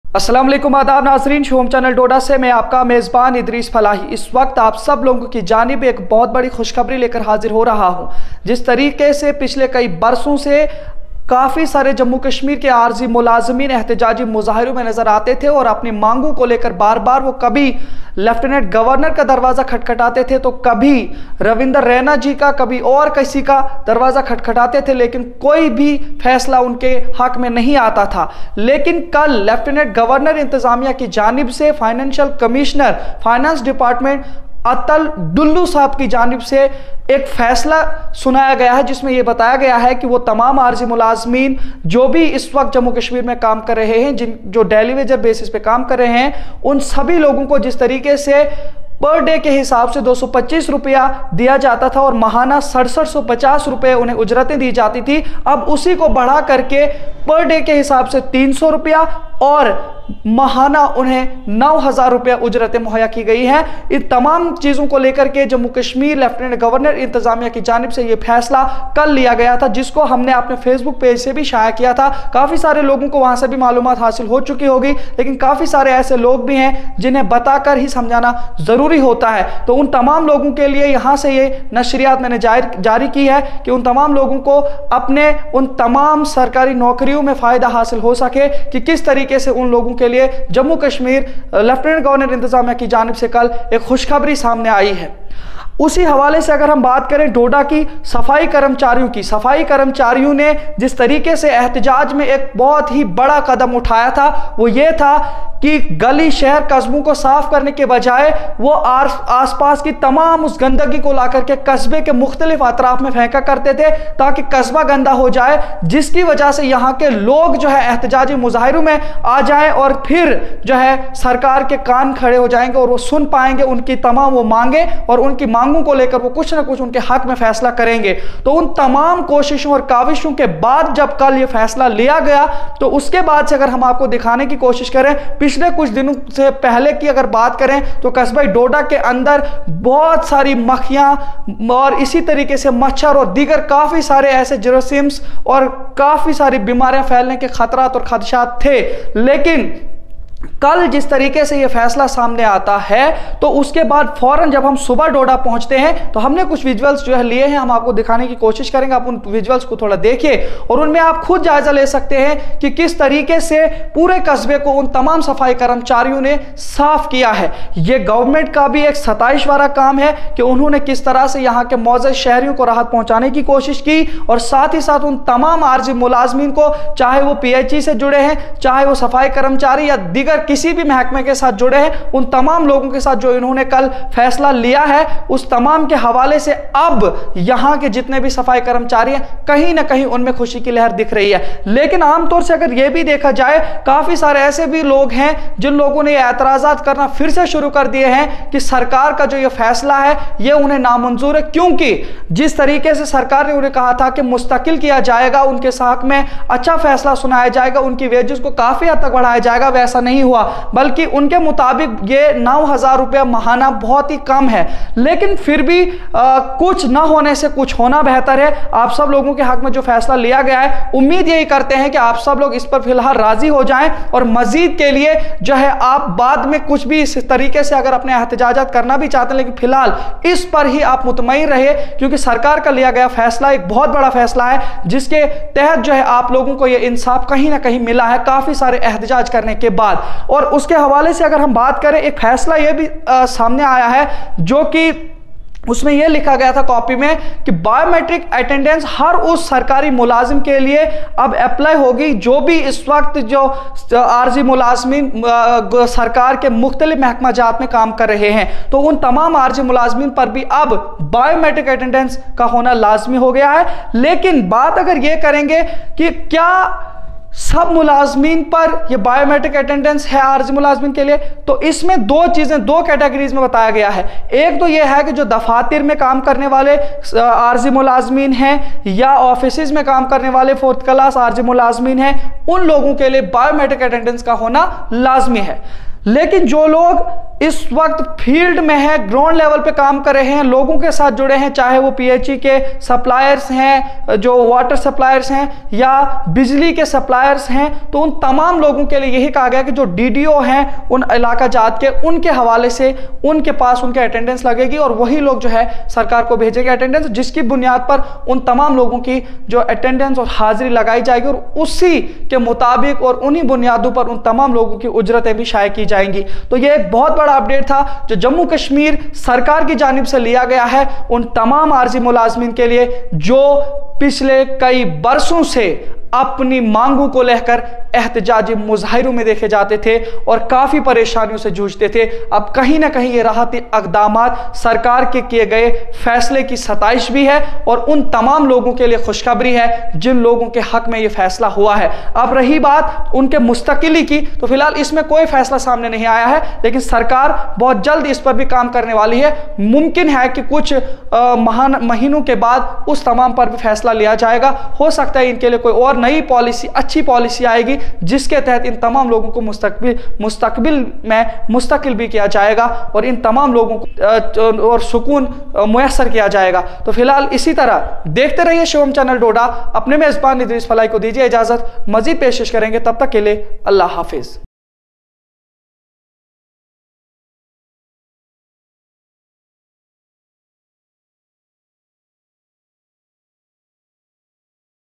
DETAILED NEWS ABOUT DAILY WAGERS .( how to apply bio metric attendance on these daily wager).
DETAILED NEWS ABOUT DAILY WAGERS .( how to apply bio metric attendance on these daily wager).Idrees Falahi official.24/04/2022